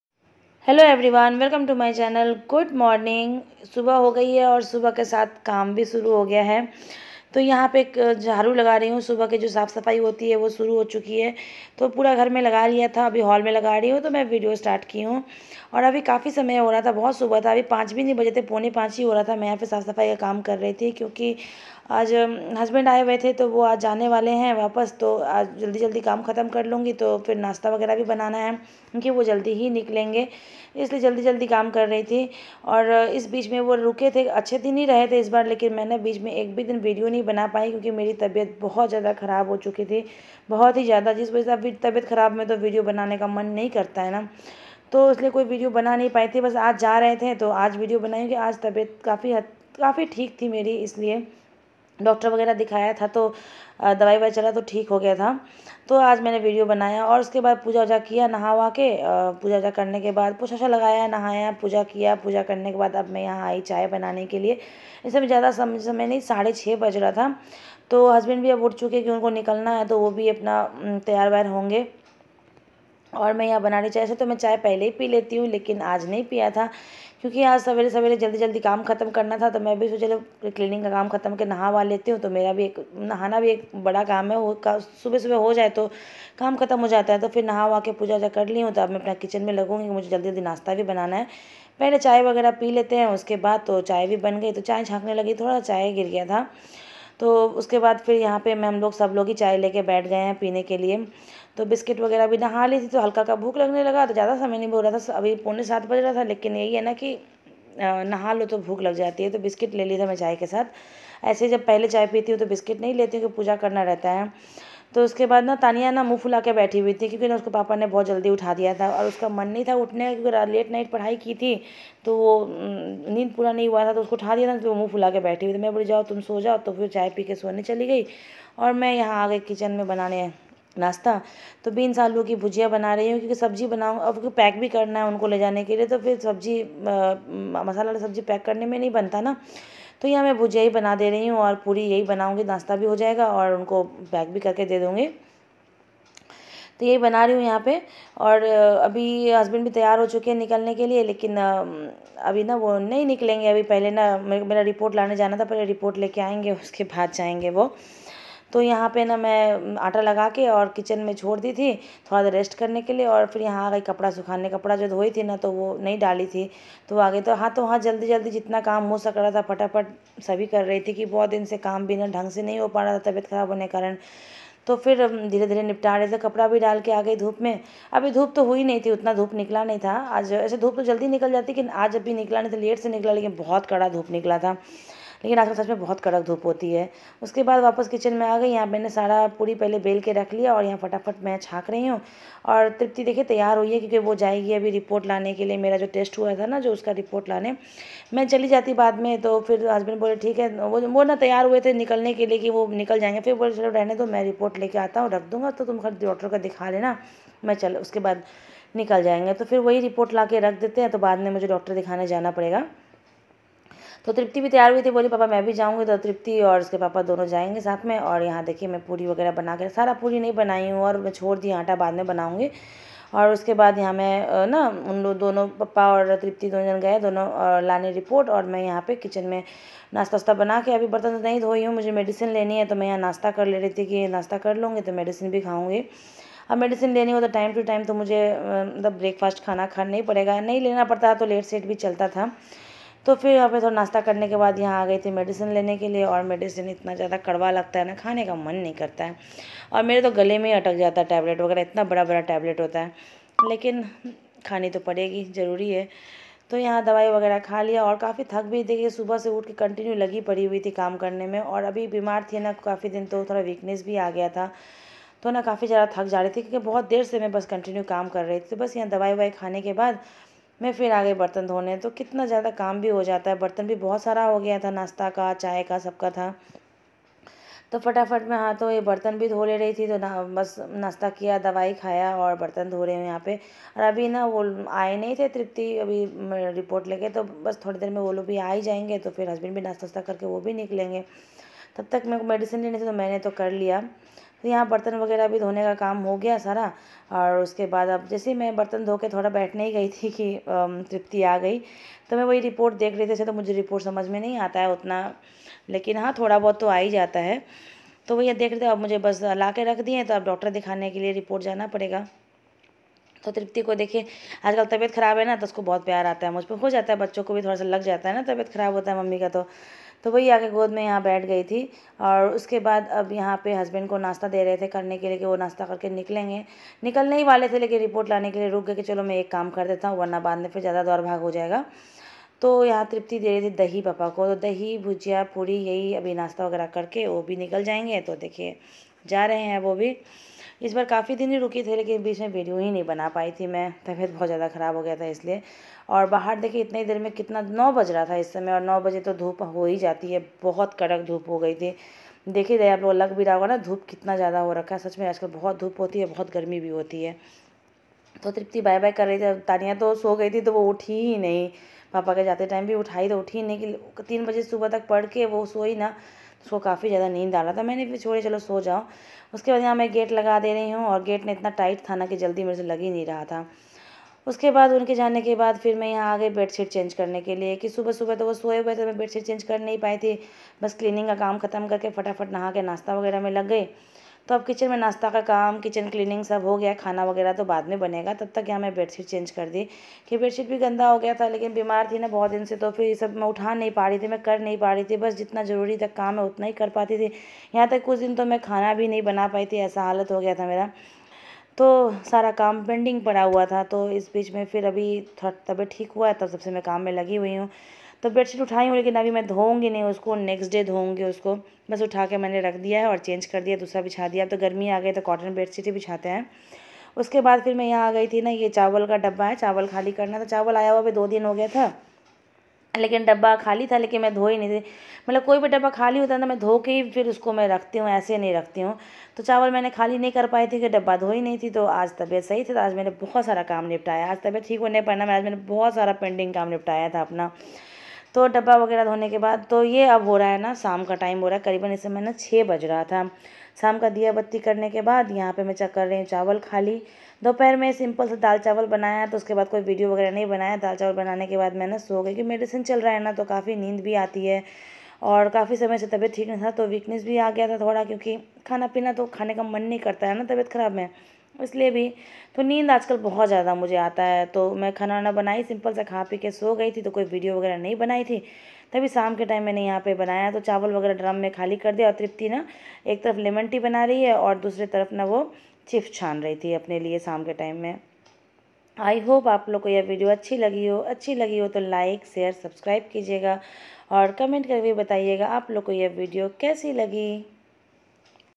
खुदका तबियत खराब हो जाए तो सारा काम पड़ा रह जाता है🥲 Indian housewife daily routine vlog❤️
खुदका तबियत खराब हो जाए तो सारा काम पड़ा रह जाता है🥲इंडियन housewife daily routine vlog❤️